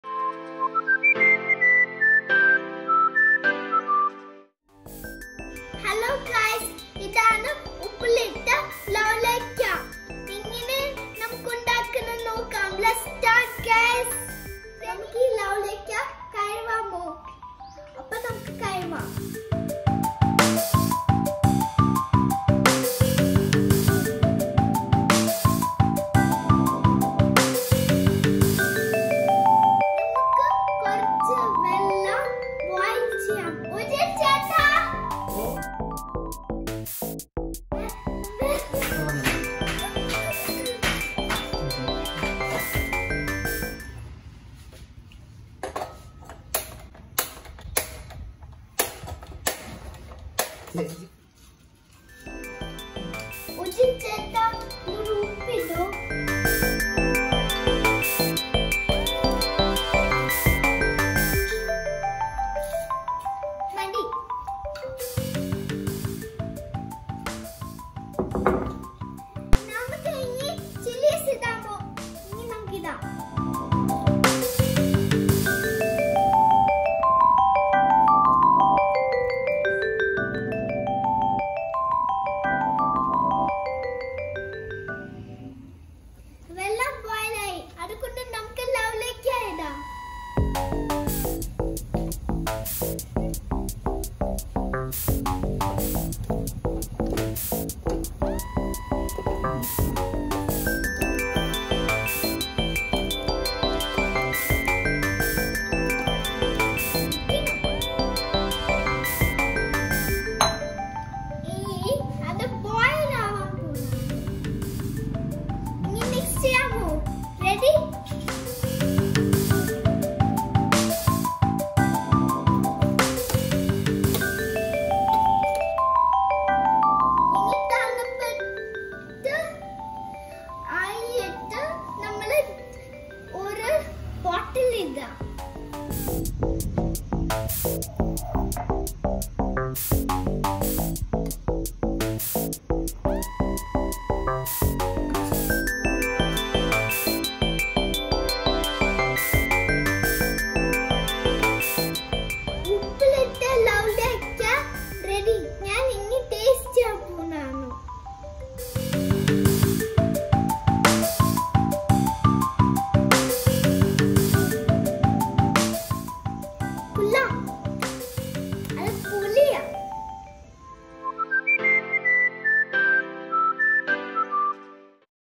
Lololikka Pickling and Bottling with Natalia | Kinsfolk Passions
In this video, Natalia pickles the harvested lololikkas. Turn captions on for english subtitles.